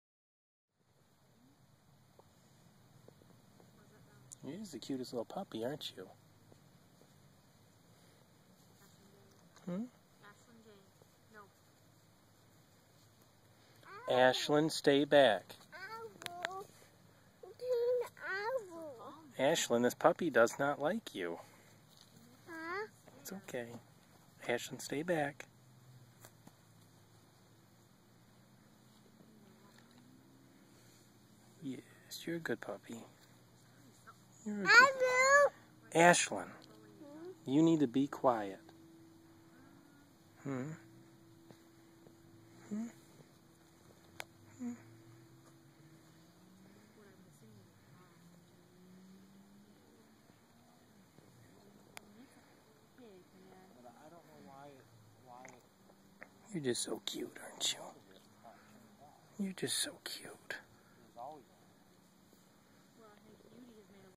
What would you name this pup?
A dog from next door has taken a liking to me. Cute little girl.